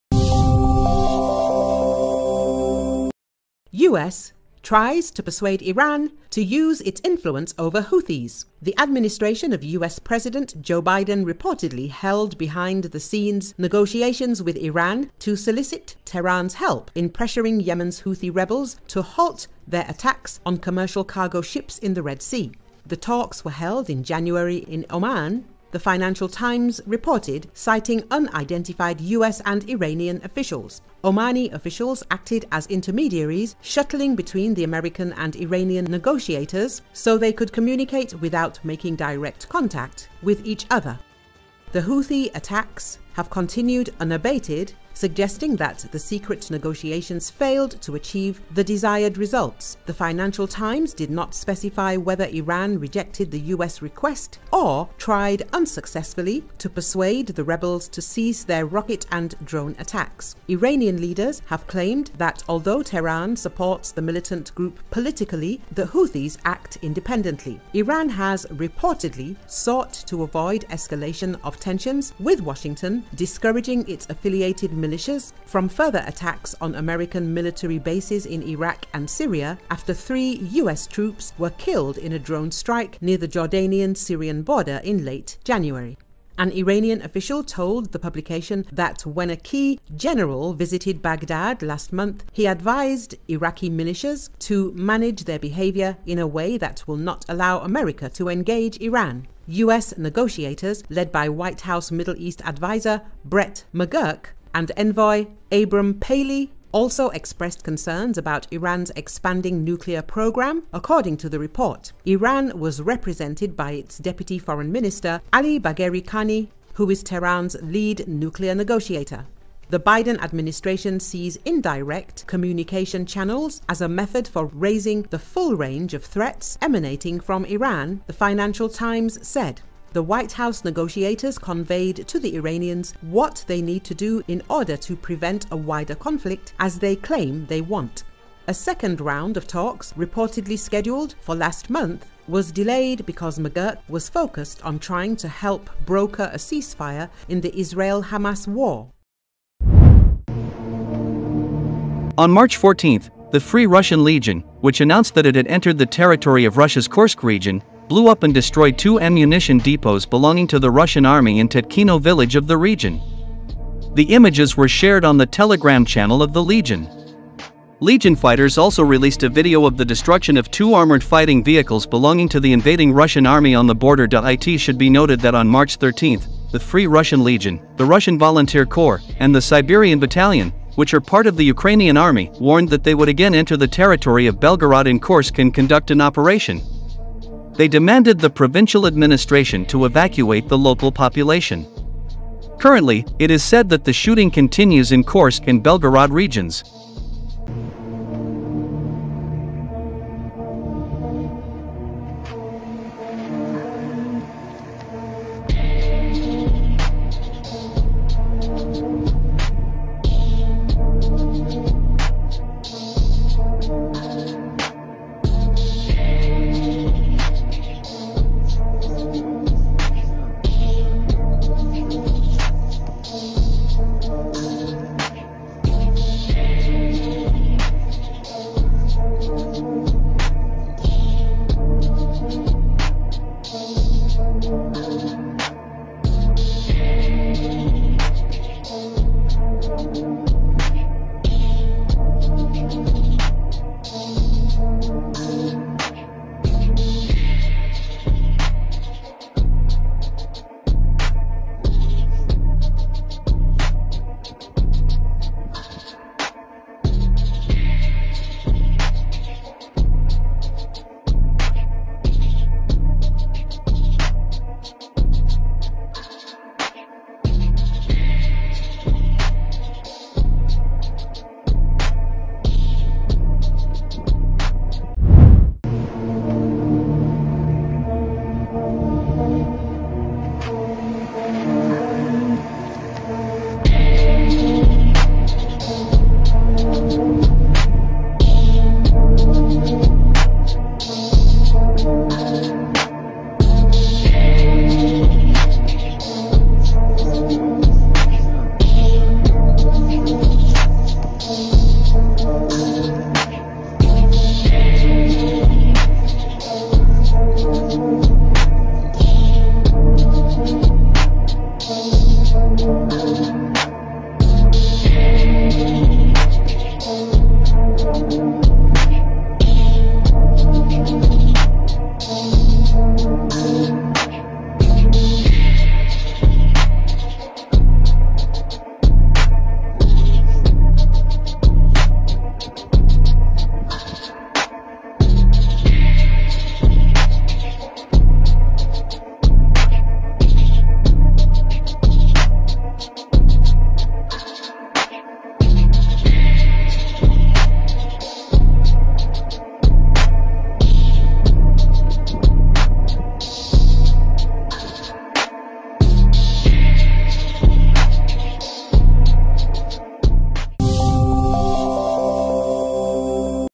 US seeks to persuade Iran to use its influence over Houthis
The administration of US President Joe Biden reportedly held behind-the-scenes negotiations with Iran to solicit Tehran’s help in pressuring Yemen’s Houthi rebels to halt their attacks on commercial cargo ships in the Red Sea.
The talks were held in January in Oman, the Financial Times reported , citing unidentified US and Iranian officials. Omani officials acted as intermediaries, shuttling between the American and Iranian negotiators so they could communicate without making direct contact with each other.
The Houthi attacks have continued unabated, suggesting that the secret negotiations failed to achieve the desired results. The Financial Times   did not specify whether Iran rejected the US request or tried unsuccessfully to persuade the rebels to cease their rocket and drone strikes. Iranian leaders have claimed that although Tehran supports the militant group politically, the Houthis act independently.
Iran has reportedly sought to avoid escalation of tensions with Washington, discouraging its affiliated militias from further attacks on American military bases in Iraq and Syria after three US troops were killed in a drone strike near the Jordanian-Syrian border in late January. An Iranian official told the publication that when a key general visited Baghdad last month, he advised Iraqi militias to “manage their behavior in a way that will not allow America to engage Iran.”
US negotiators – led by White House Middle East adviser Brett McGurk and envoy Abram Paley – also expressed concerns about Iran’s expanding nuclear program, according to the report. Iran was represented by its deputy foreign minister, Ali Bagheri Kani, who is Tehran’s lead nuclear negotiator.
The Biden administration sees indirect communication channels as “a method for raising the full range of threats emanating from Iran,” the Financial Times   said. The White House negotiators conveyed to the Iranians “what they need to do in order to prevent a wider conflict, as they claim they want.”
A second round of talks, reportedly scheduled for last month, was delayed because McGurk was focused on trying to help broker a ceasefire in the Israel-Hamas war.